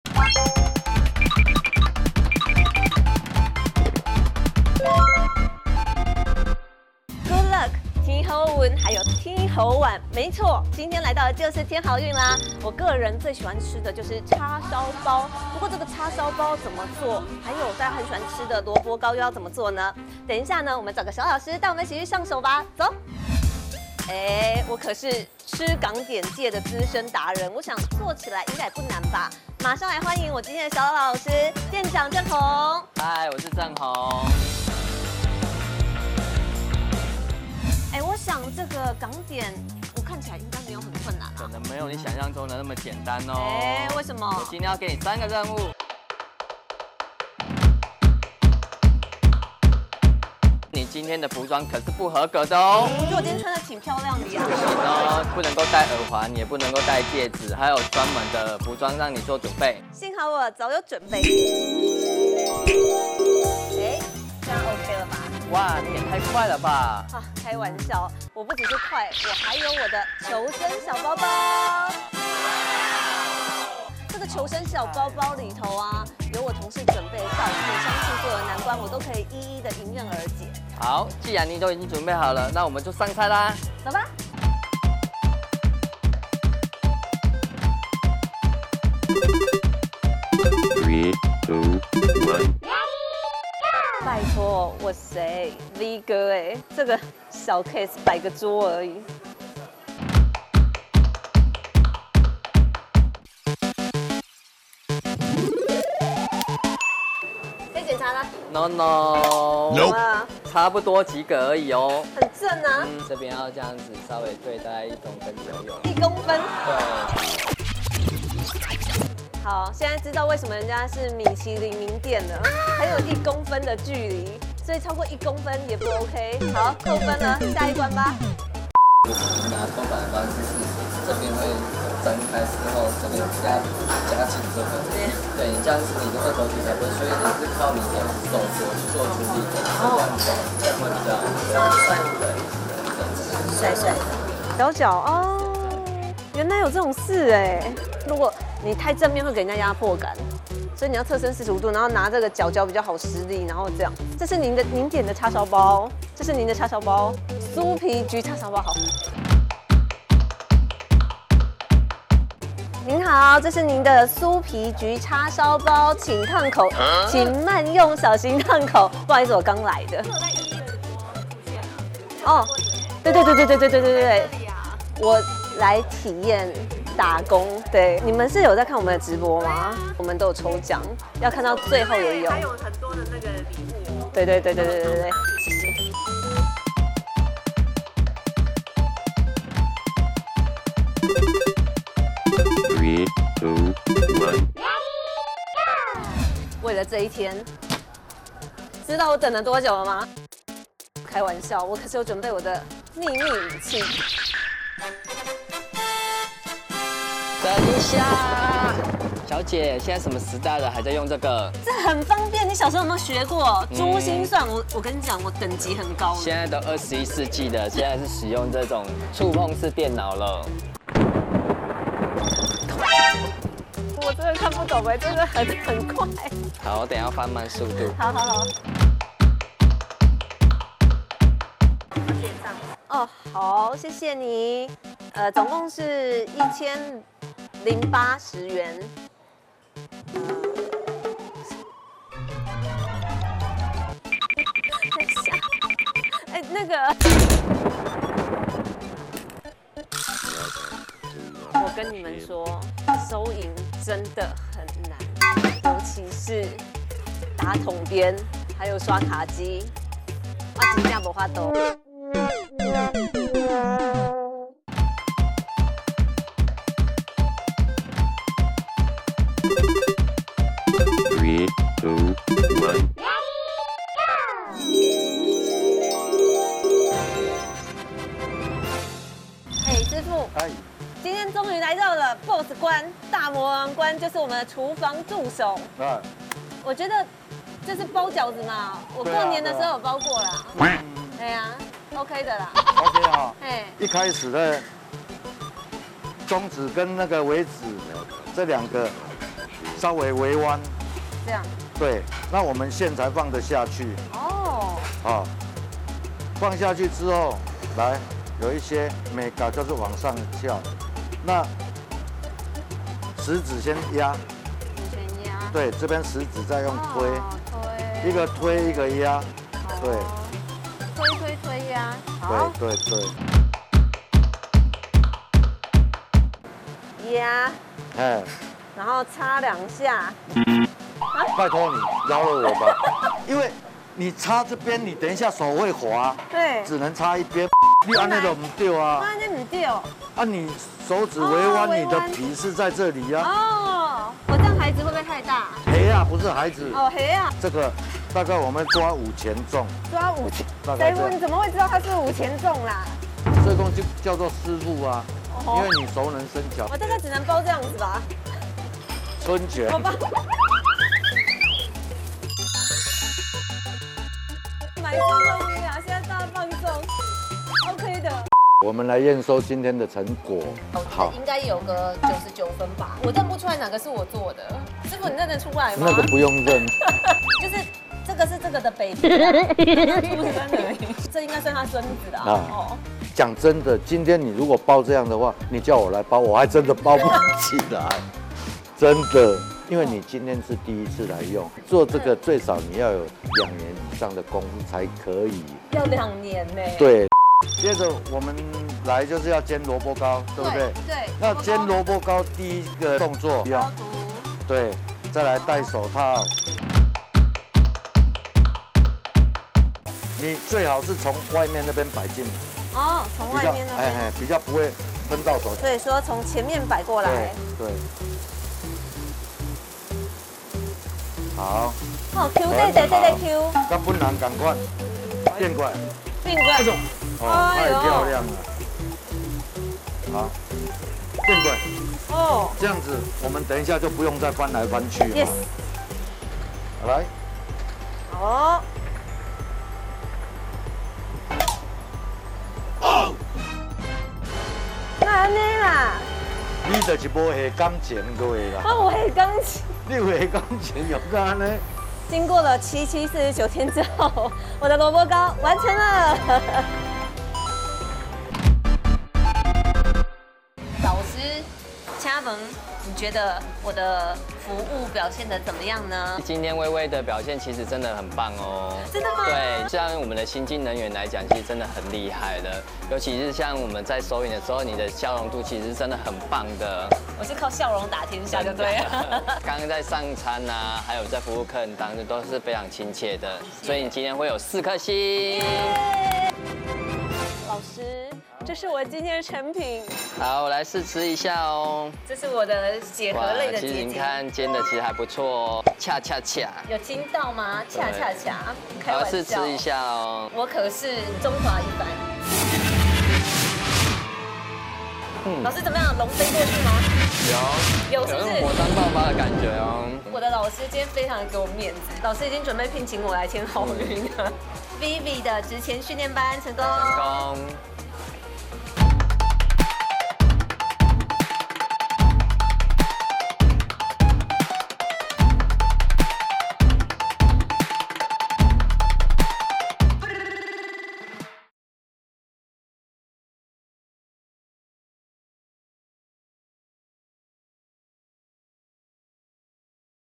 黃若薇第一次做鮮蝦餃就上手？《添好運》神秘廚房獨家曝光！｜薇薇職場先修班EP01
1111發言人 黃若薇Vivi的薇薇職場先修班開課囉，帶大家一起進入各行各業，由職場菁英們為大家帶路，想找尋你的命定職業嗎？ 讓薇薇來幫你解惑~
第一集來到了”米其林餐廳”『添好運』，外場服務人員、結帳收銀員、廚房助手，工作內容第一次完整大公開，一起來看看吧！！

【新鮮人精選職缺看這裡👌】
✨外商職缺   點這裡👇

【同場加映🍀】
✨找工作送１萬  事業荷包兩得意👇